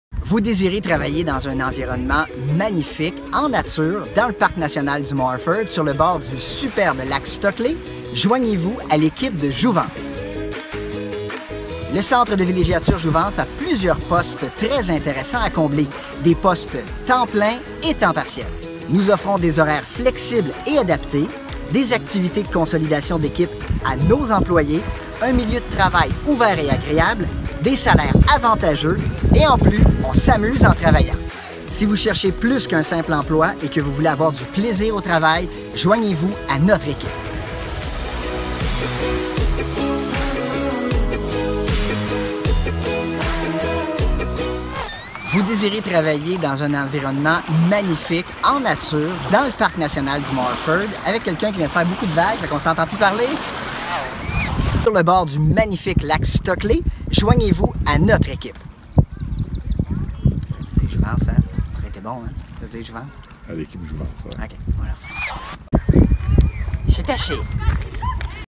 Le Centre de Villégiature recrute
Le Centre de Villégiature Jouvence a plusieurs beaux postes de disponibles.  Découvrez l'environnement de travail que vous réserve ce magnifique site dans le parc du Mont Orford et contactez-nous pour postuler à .